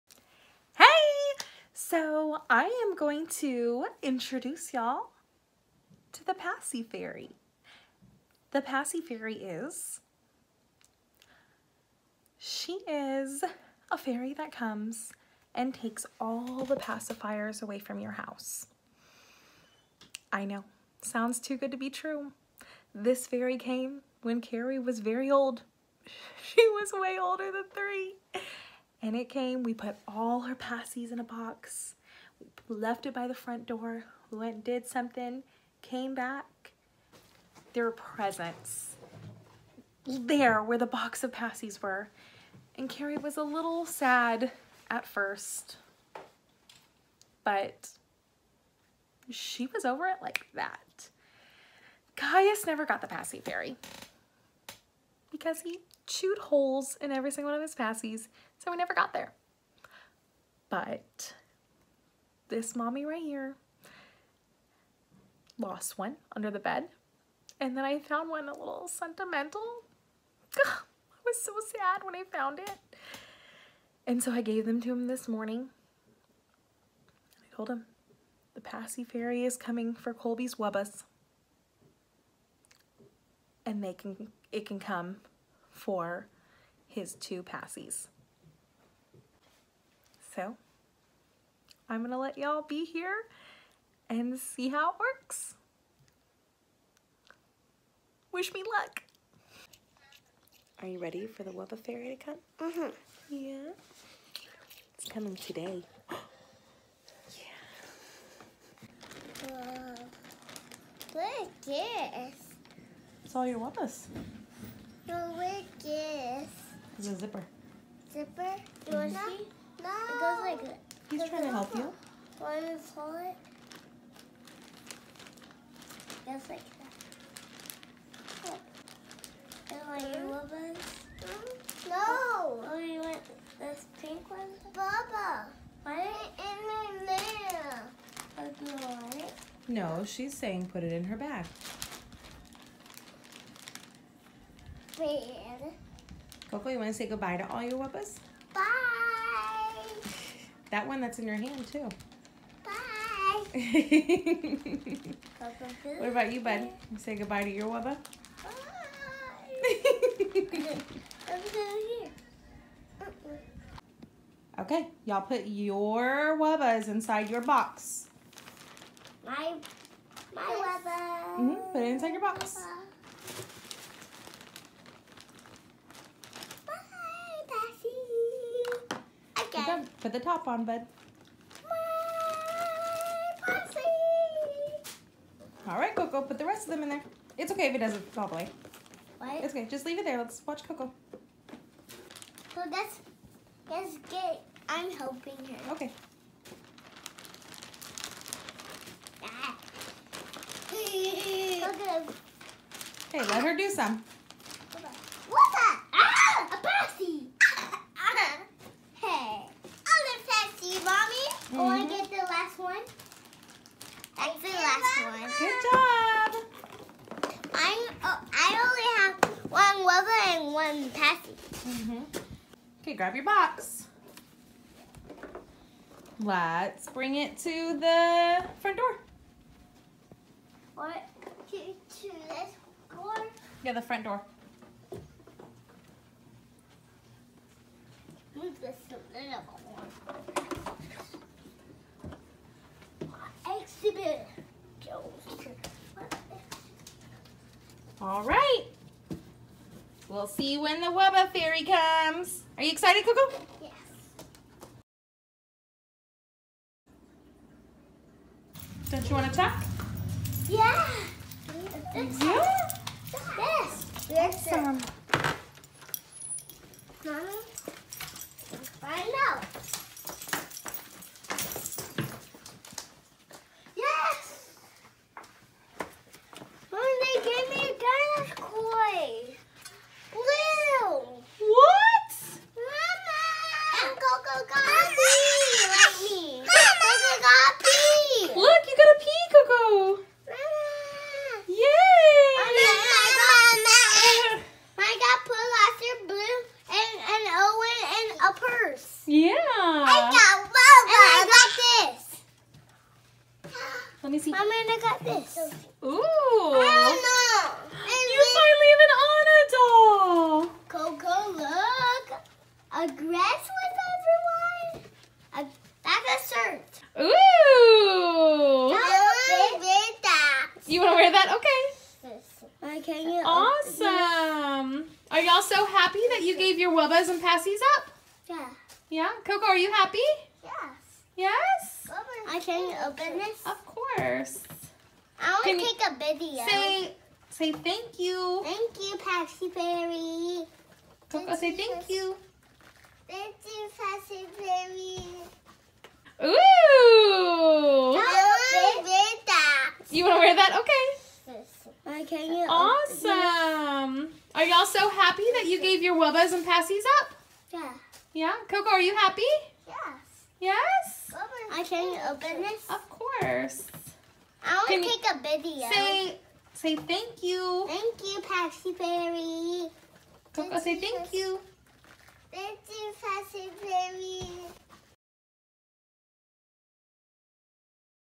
The pacifier fairy came. How to get rid of a pacifier the easy way
Join the two littlest Joseph on their journey to big kid life! The Paci Fairy is a creature that came a long time ago for Kerri and it’s Kaius’ and Kolby’s turn!